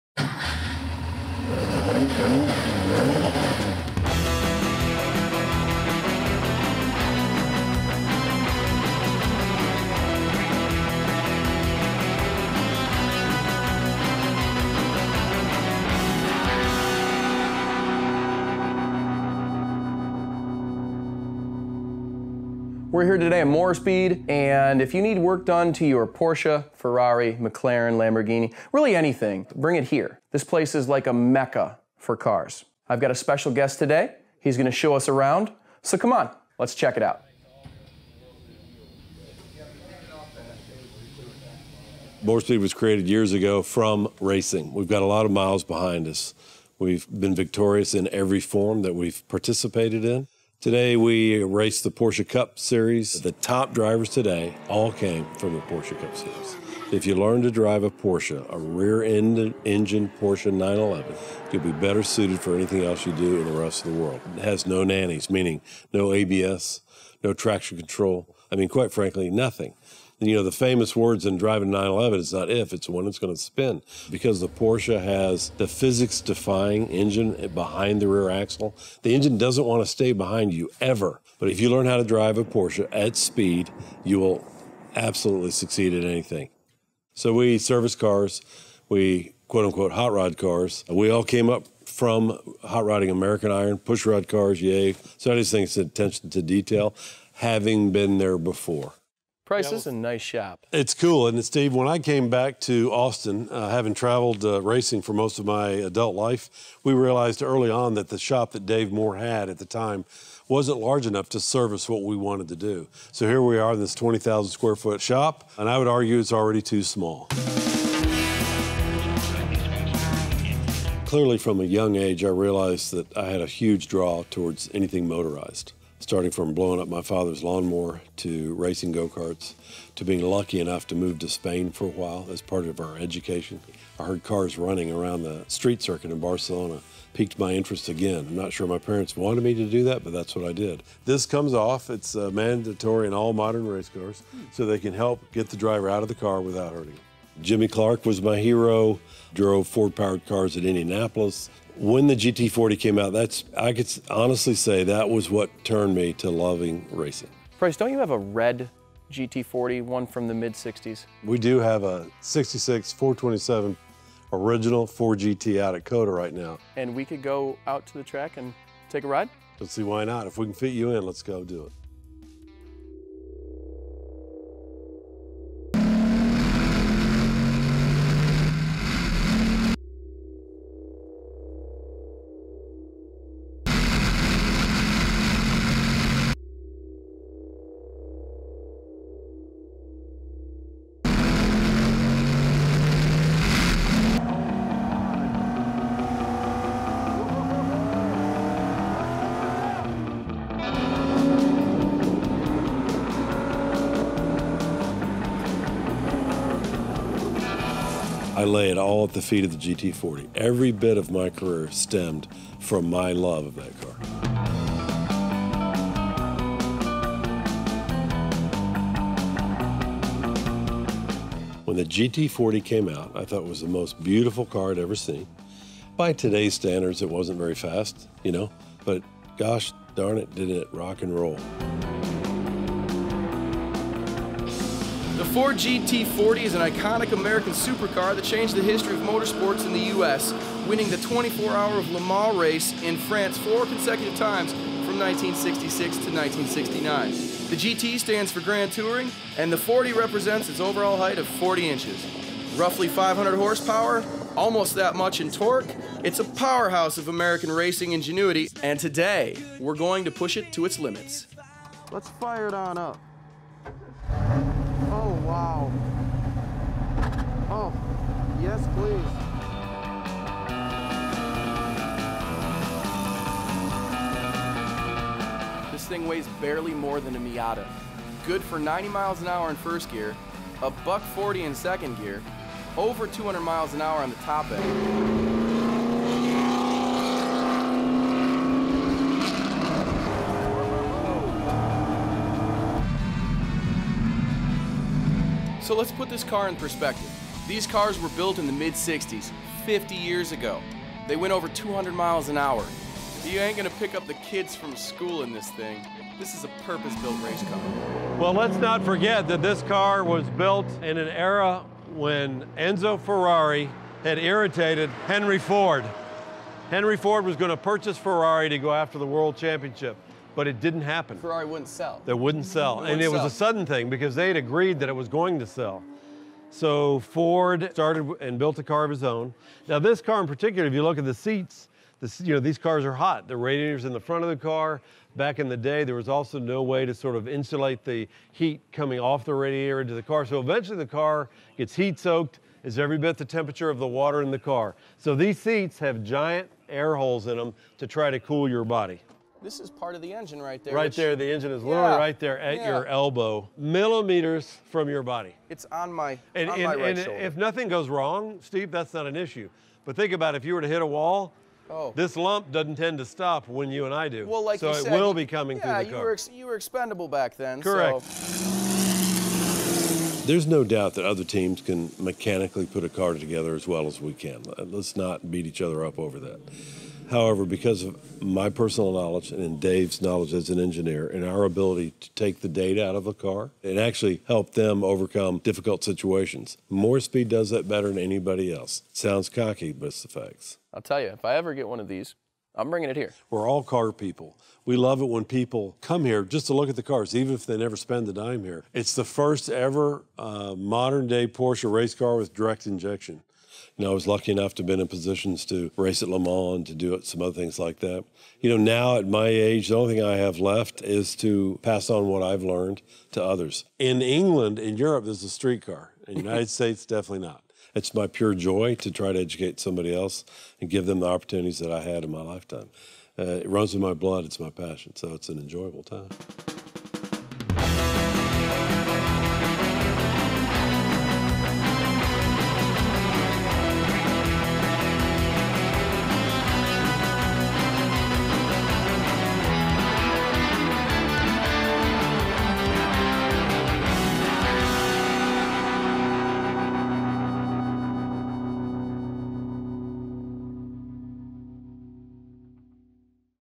COTA TRACK DAY IN A 1966 FORD GT40
KeepAustinGeared. We join 24 Hours of LeMans winner Price Cobb and the Moorespeed team as they take out the 1966 427 Mk II Ford GT40 for a few hot laps at Circuit of the Americas.